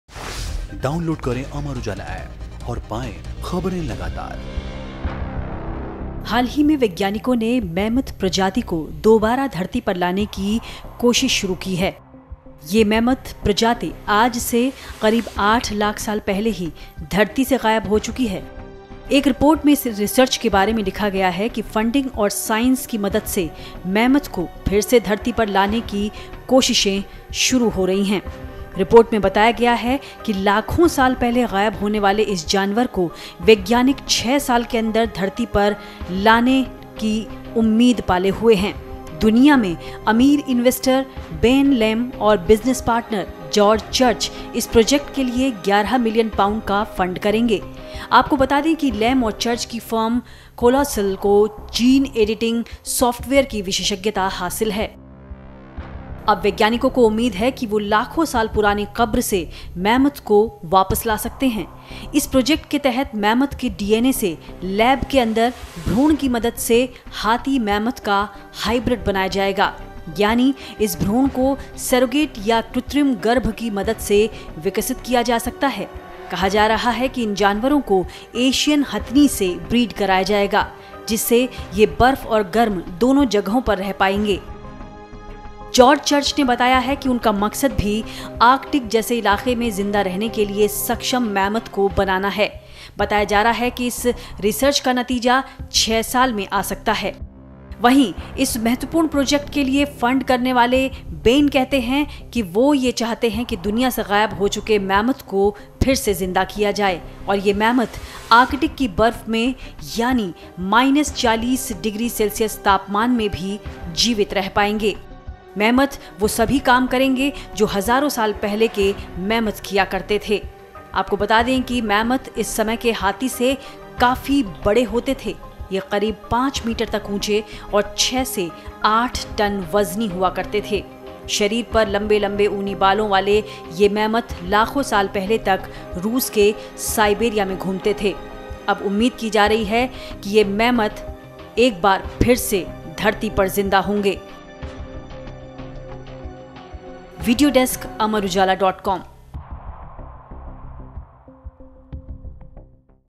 Mammoth प्रजाति को फिर धरती पर लाने की कोशिशें शुरू
हाल ही में वैज्ञानिकों ने ये घोषणा की है कि लाखों साल पहले साइबेरिया में घूमने वाली मैमथ प्रजाति को एक बार फिर जिंदा किया जाएगा। जानिए वैज्ञानिकों ने क्या कहा है।